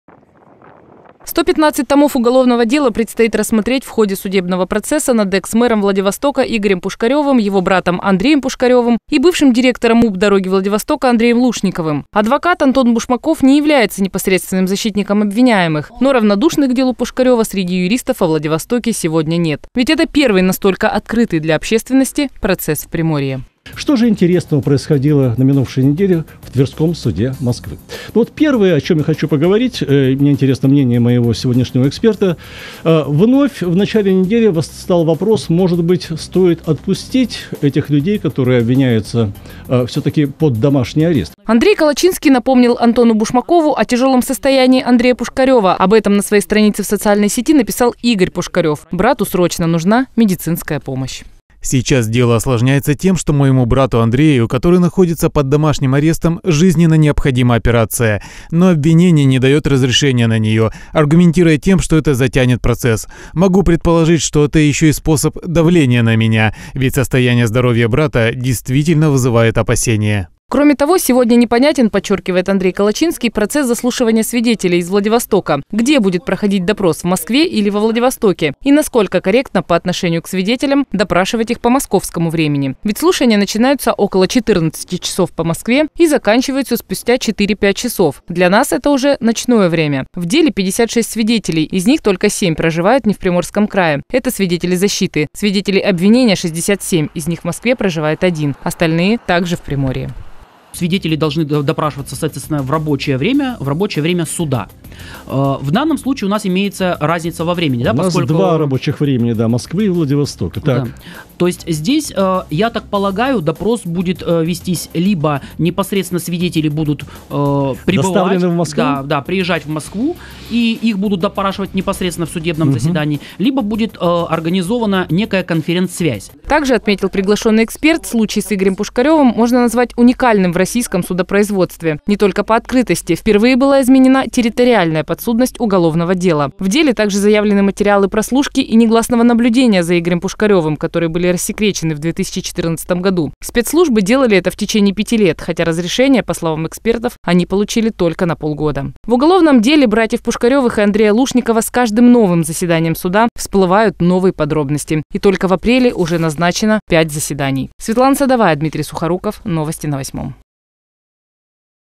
СвободуПушкареву
Сто пятнадцать томов уголовного дела предстоит рассмотреть в ходе судебного процесса над экс-мэром Владивостока Игорем Пушкаревым, его братом Андреем Пушкаревым и бывшим директором МУП Дороги Владивостока Андреем Лушниковым. Адвокат Антон Бушмаков не является непосредственным защитников обвиняемых, но равнодушных к делу Игоря Пушкарева среди юристов во Владивостоке сегодня  нет.Ведь это первый настолько открытый для общественности процесс в Приморье